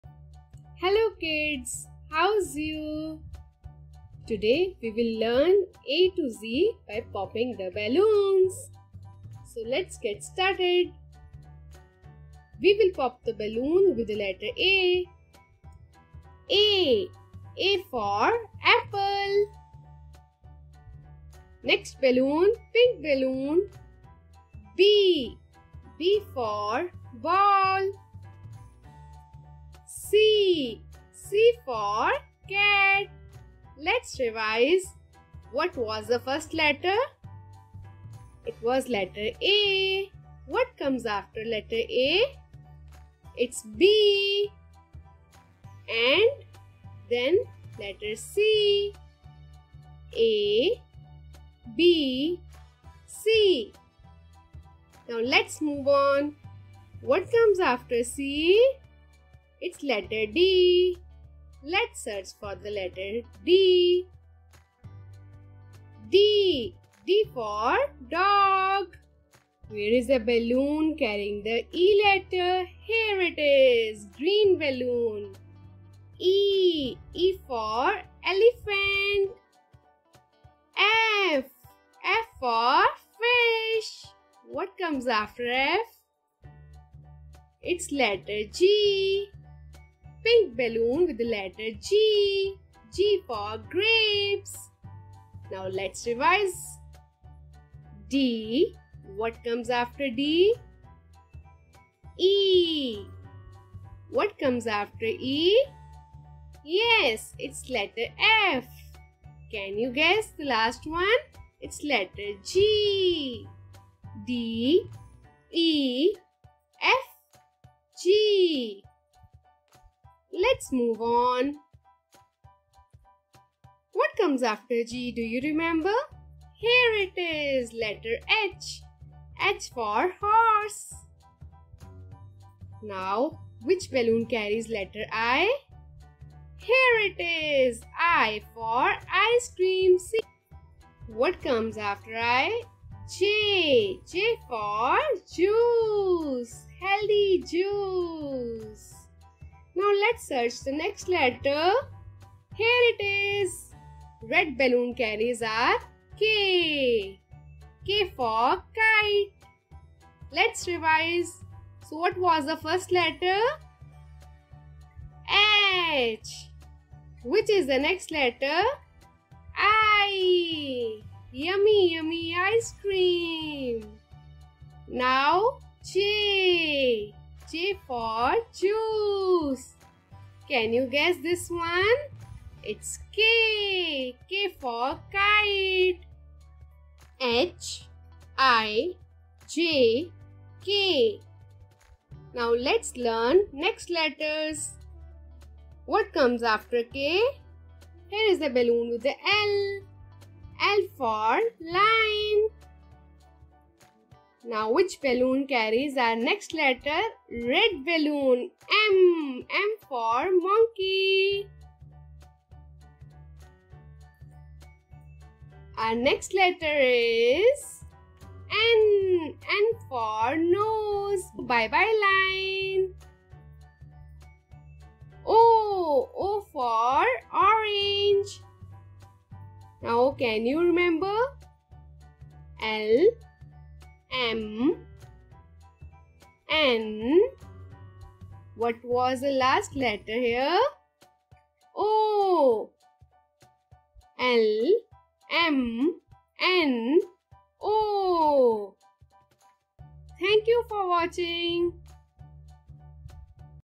ABC Balloon Popping Fun: Learn Your Letters in a Unique and Engaging Way!
In this fun and engaging video, children will learn the small abc by popping balloons! Our unique teaching method makes learning the alphabet exciting and memorable. As each balloon pops, children will be introduced to a new letter and its corresponding sound. Our experienced instructors carefully designed this lesson to improve letter recognition and early reading skills in children. By making learning fun and exciting, we encourage children to explore their creativity and develop a lifelong love of learning. Our "ABC Balloon Popping Fun" video is perfect for teachers, parents, and homeschoolers who are looking for an innovative and effective way to teach children the alphabet. With this video, children will have a blast while learning all of the small abc! Don't wait any longer! Start your child's educational journey today!